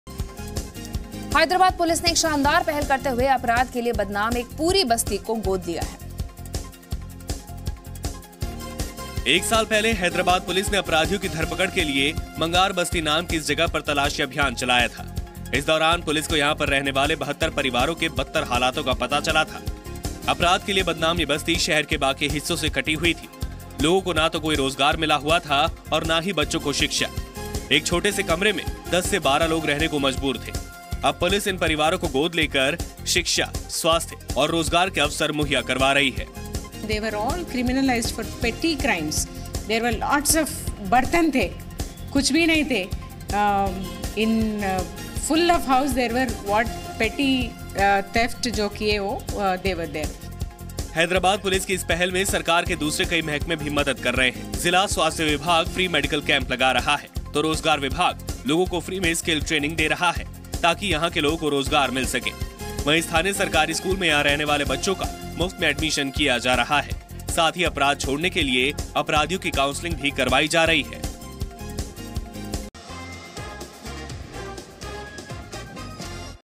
Morning Breaking: Hyderabad Police adopt slum with several history-sheeters
Hyderabad Police adopted a slum with around 72 history-sheeters, to help them overcome their bad image. Watch this video to know more.